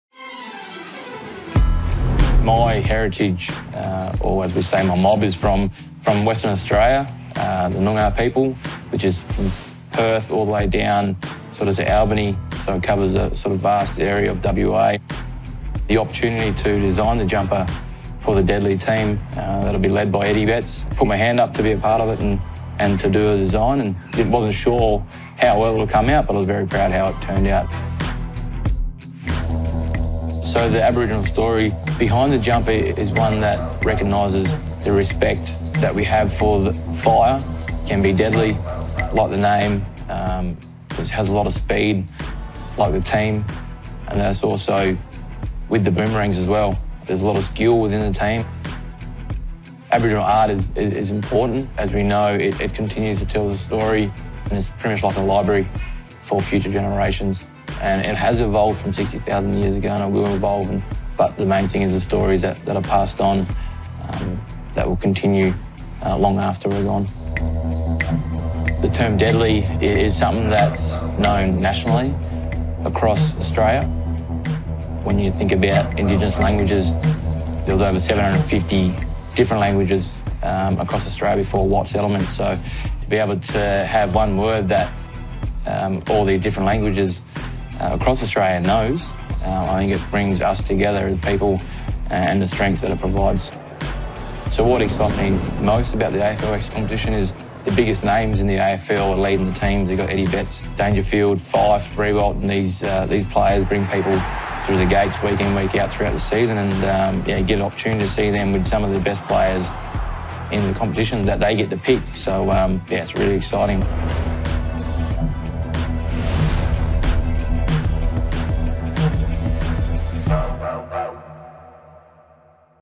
Jetta's influential hand in 'Deadly' indigenous guernsey | AFLX | 2019 | AFL
Melbourne defender Neville Jetta explains the cultural and historical significance of the AFLX team name and guernsey.